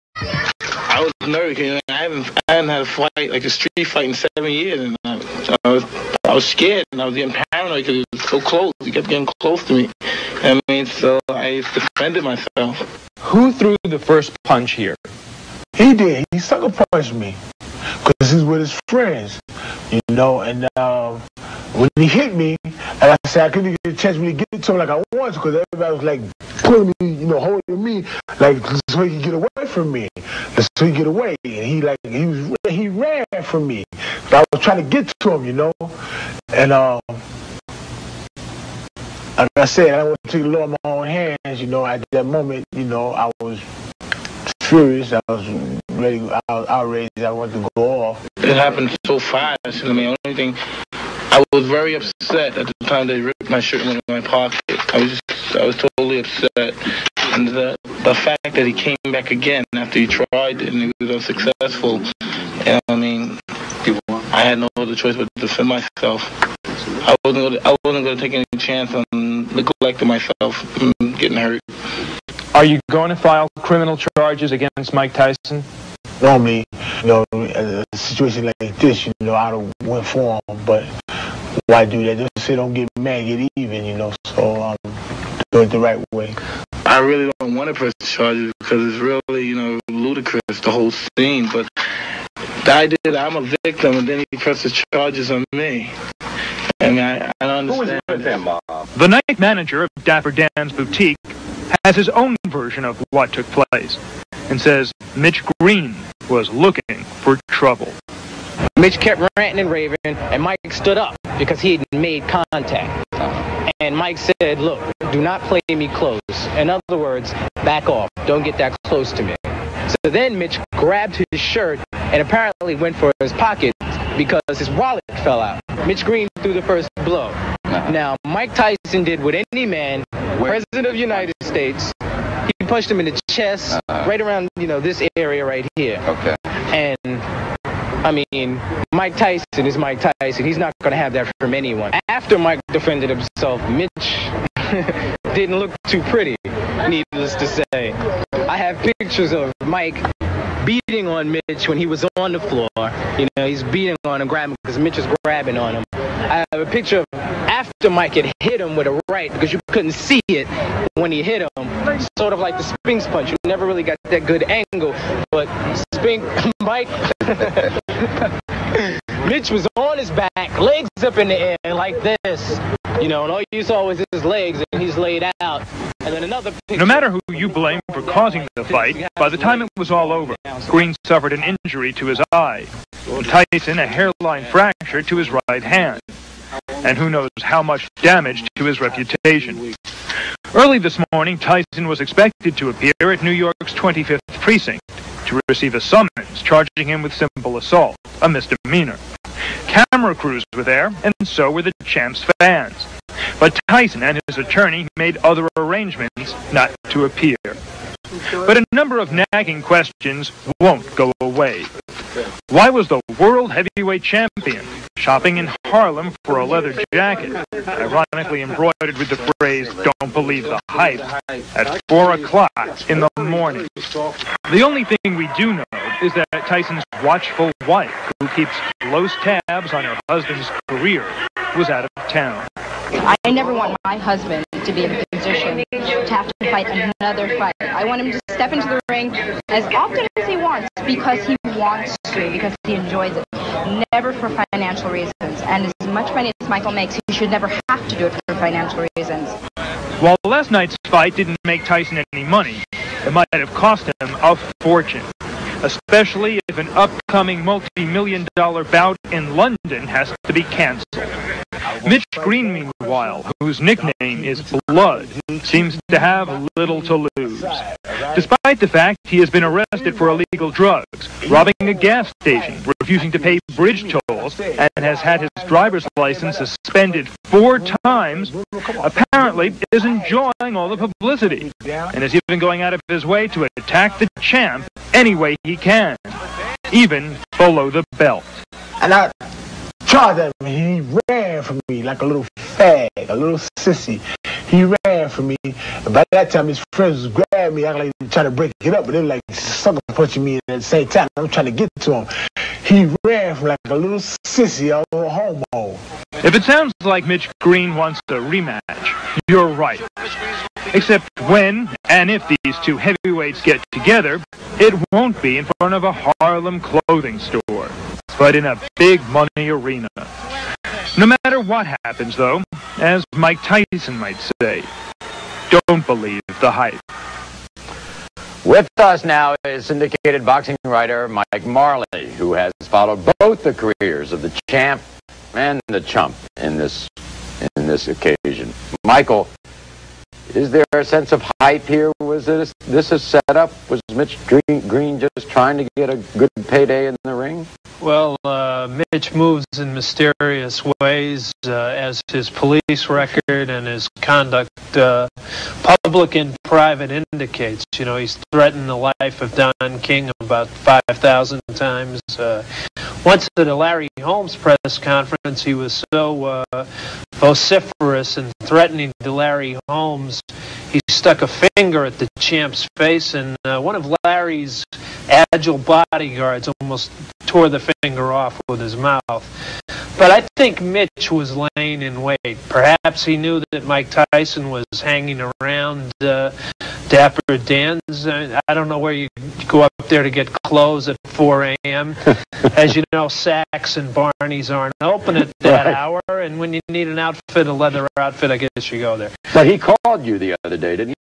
Mike Tyson and Mitch Green Talks about their street fight
After Mike Tyson knocked out Mitch Green on boxing ring, They later met on street and a fight erupted, Mike Tyson disfigured Mitch Green with a heavy Punch.